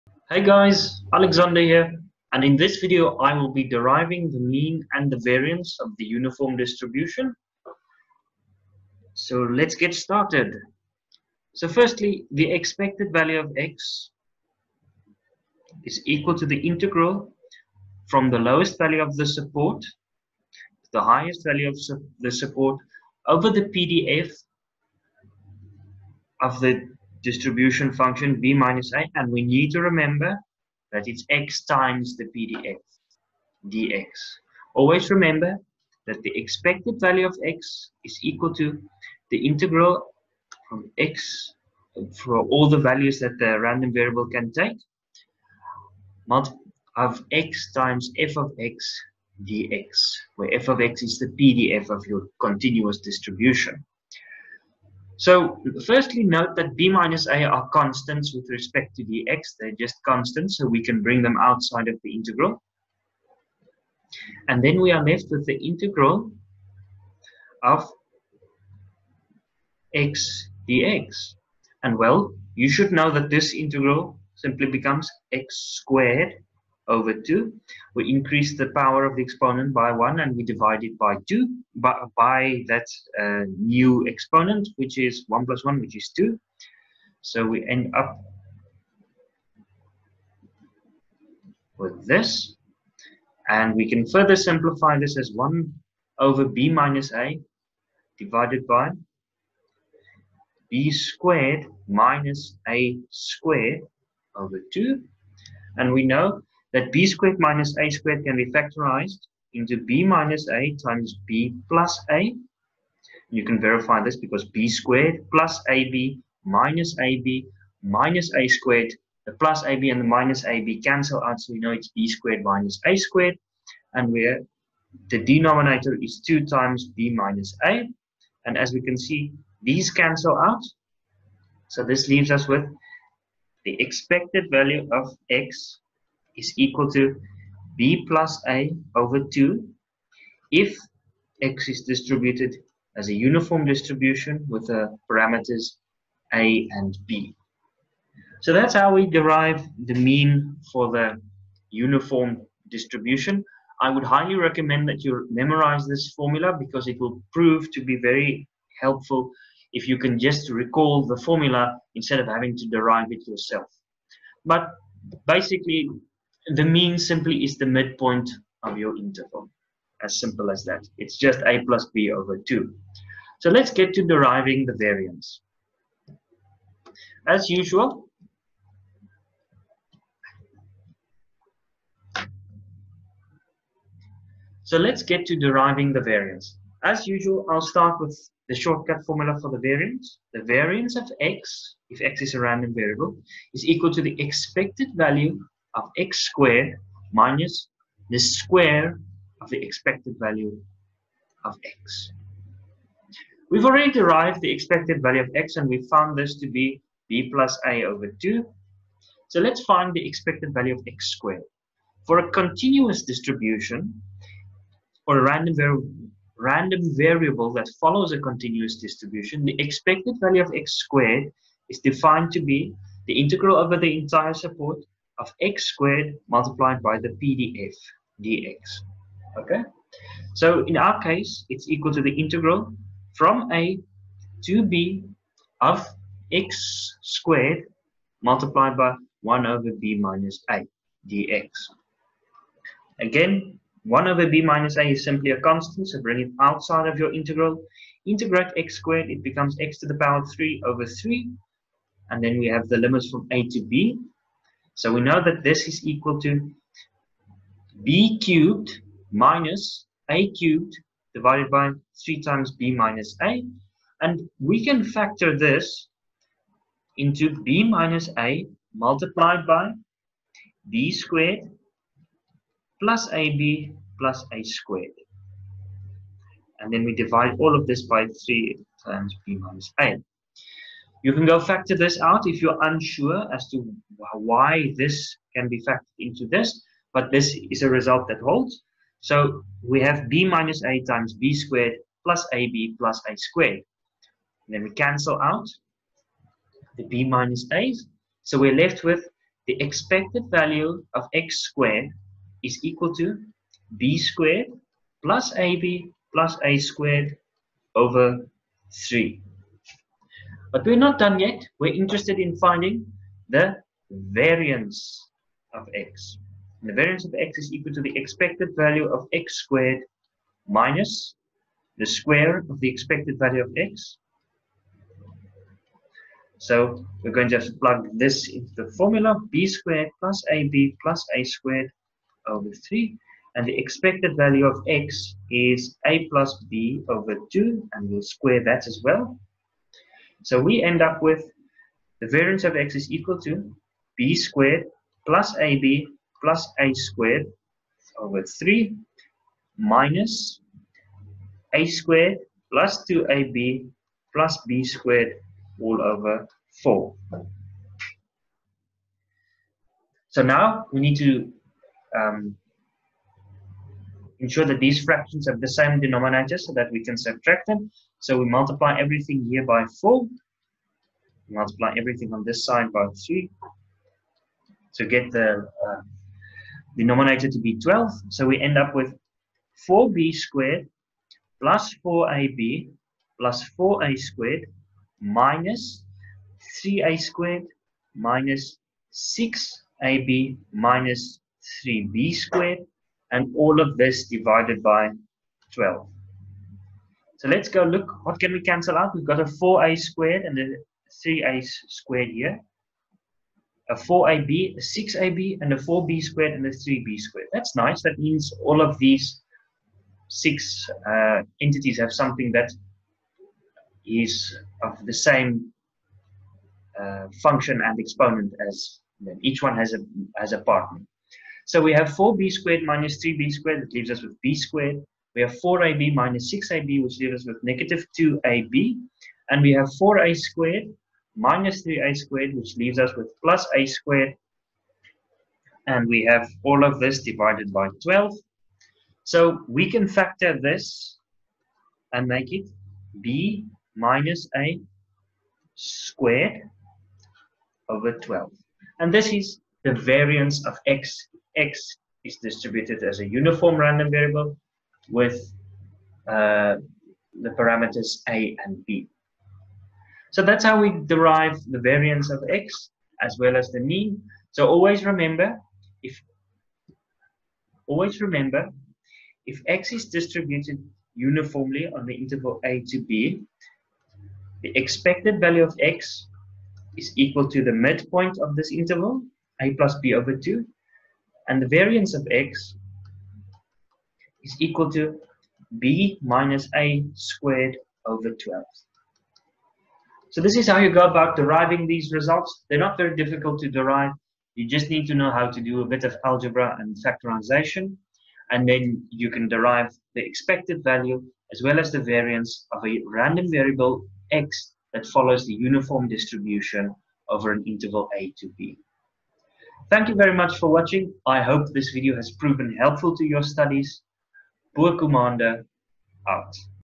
Uniform Distribution Mean and Variance Proof
In this video I provide the derivations of the mean and variance of the Continuous Uniform Distribution.